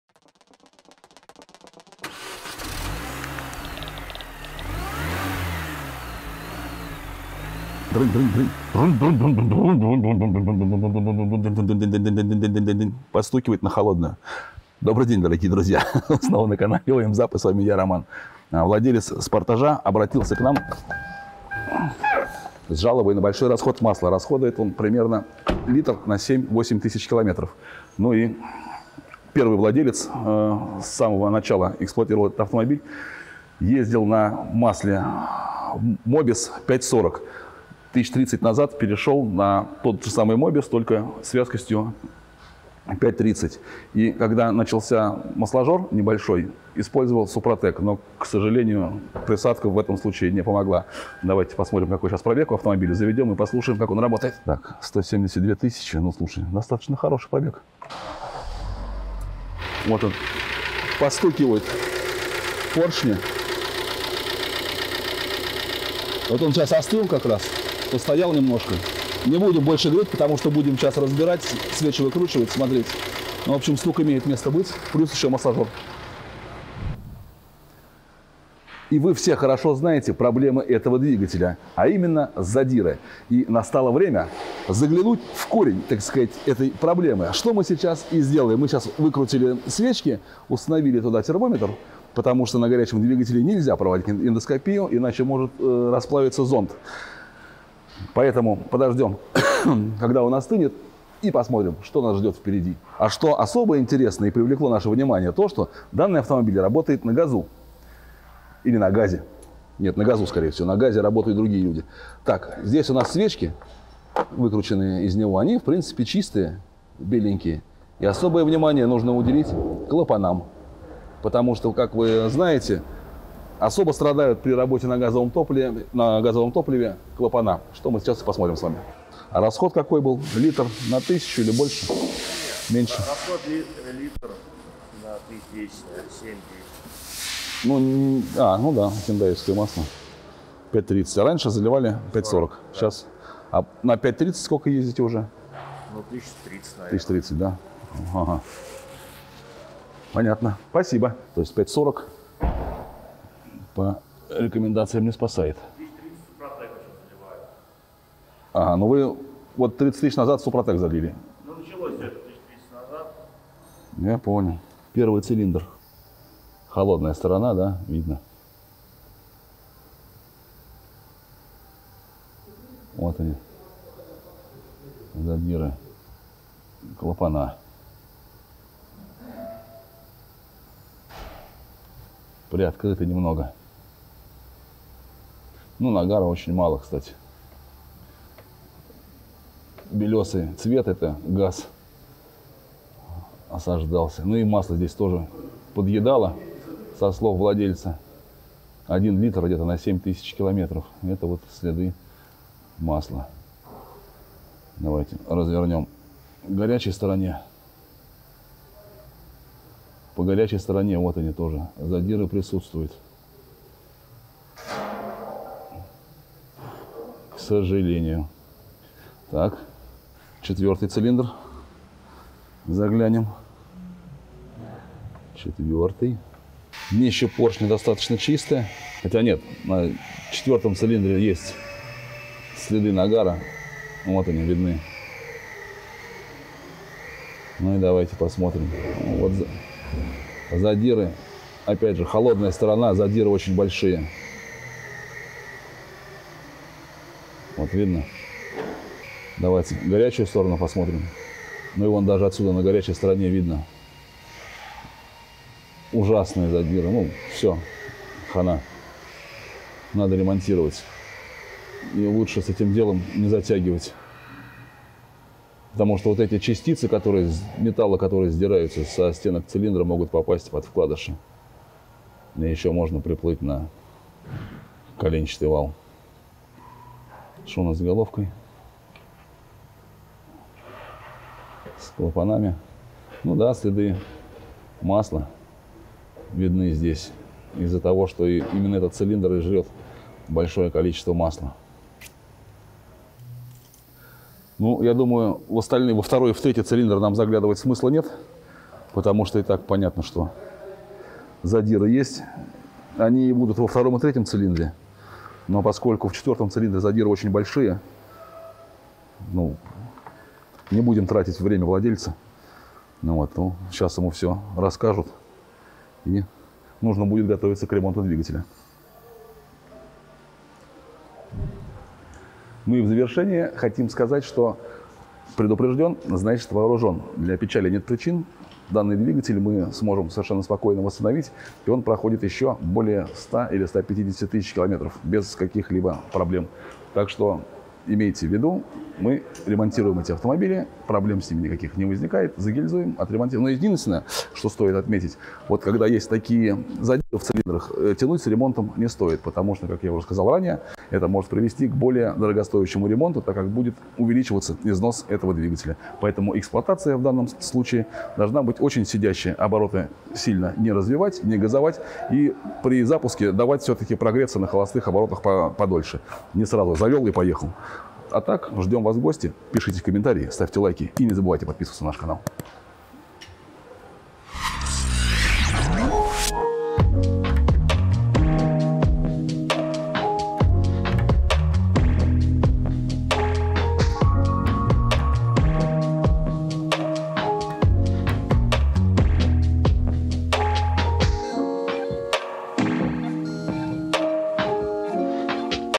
Задиры в цилиндрах G4KD 2.0 Kia Sportage. Супротек не помог.
Задиры в цилиндрах G4KD Kia Sportage. Супротек не помог.

Очередной раз сталкиваемся с проблемой стука на холодную на автомобиле Kia Sportage 3 с двигателем G4KD 2.0 с пробегом 172 т.км.. Причина обращения в сервис был масложер и как мы выяснили, что сначала эксплуатации в двигателе использовали масло Хендай 5W40, потом стали заливать 5W30, а при появлении масложера залили в двигатель «панацею» СУПРОТЕК.

Но, к сожалению, все эти манипуляции не устранили ни масложер, ни появление задиров стенок цилиндров. Как бороться с задирами, мы писали неоднократно, но повторимся. Для того, чтобы устранить задиры требуется произвести капитальный ремонт двигателя с гильзовкой блока цилиндров и заменой поршней на новые, стандартного размера (мы используем исключительно оригинальные). Установка масляных форсунок при этом ремонте не обязательна, т.к. задиры появляются на автомобилях, где они установлены с завода.

После произведенного капитального ремонта двигателя, мы рекомендуем использовать масло с вязкостью 5W20, т.к. оно, по заявлению завода Хендай, является оптимальным для серии этих двигателей. Сейчас, конечно, налетят «критики» и будут утверждать, что какой-то там канал заливает 5W40. Пусть заливает, Вы никогда не узнаете, что стало с автомобилем в дальнейшем. Просто история на этом «того канала» прервется. А мы вступаем в долгосрочные отношения с нашими клиентами и при случае все расскажем и покажем.

Находимся по адресам:
👇👇👇👇👇👇👇👇👇ЖМИ КНОПКУ "ЕЩЁ"👇👇👇👇👇👇👇👇👇

Север
🕘🔛🕗 График работы: с 9 до 20 без выходных.